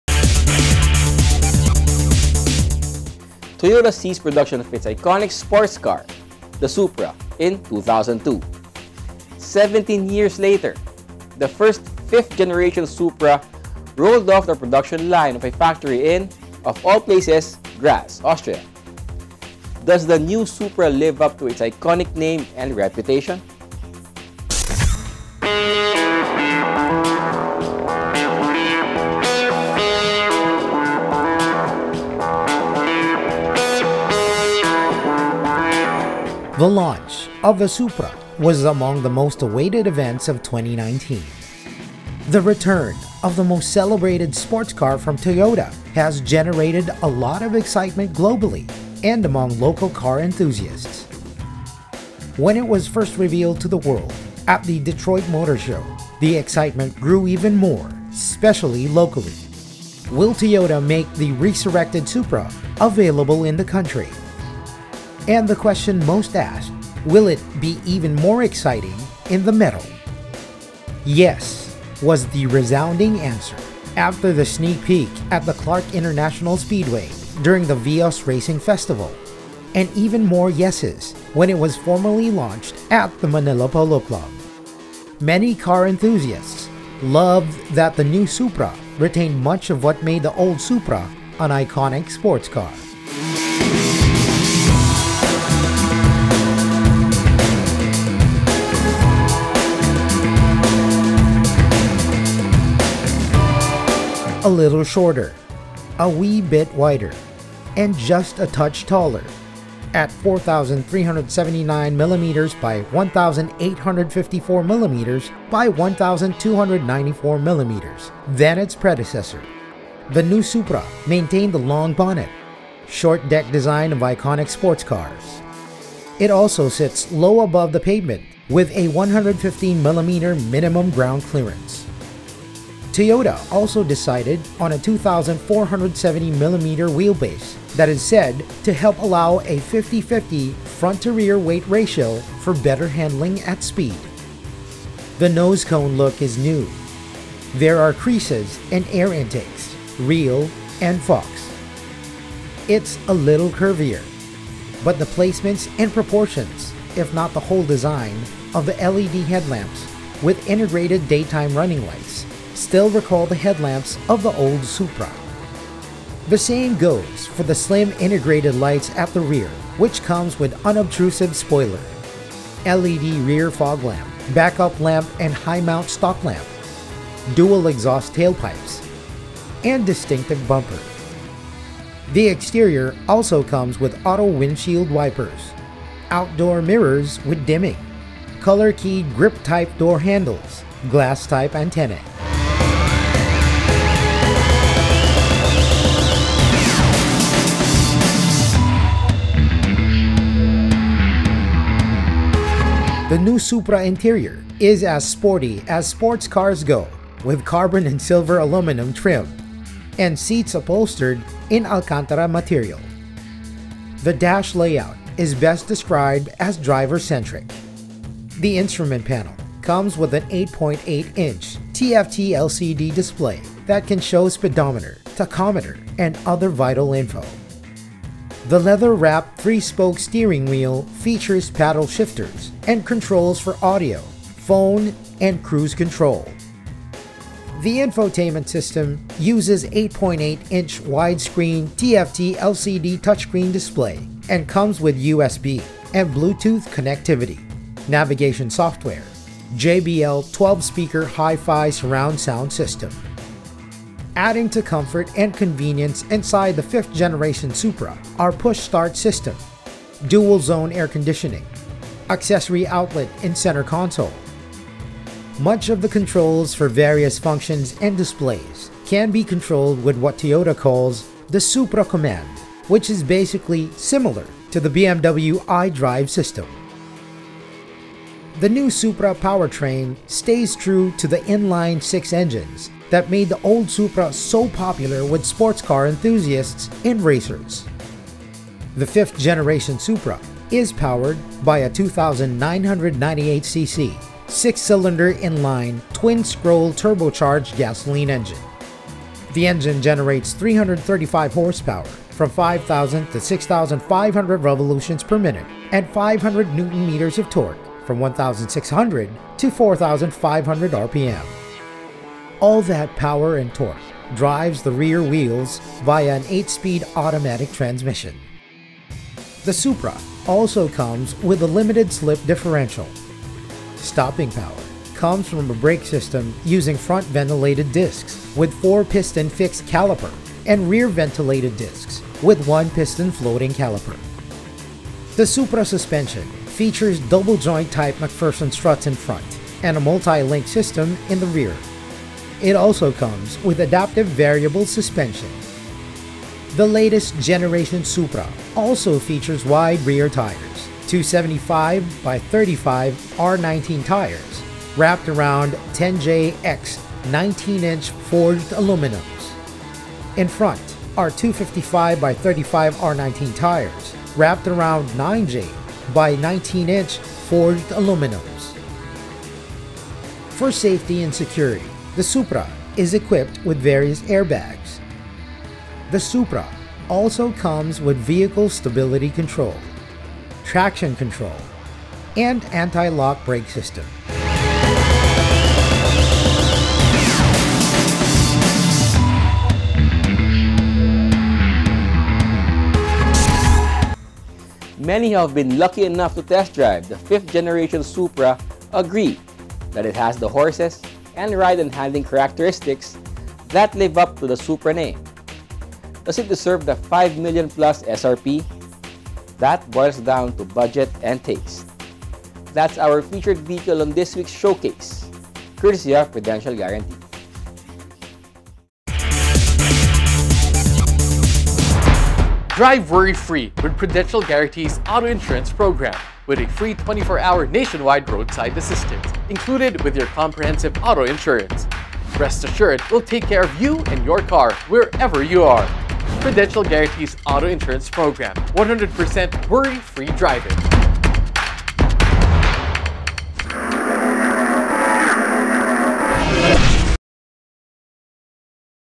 Toyota Supra 3.0L A/T | Showcase
Many who have been lucky enough to test drive the 5th generation Supra agree that it has the horses, and ride and handling characteristics that live up to the Supra name. Does it deserve the Php 5M plus SRP? That boils down to budget and taste.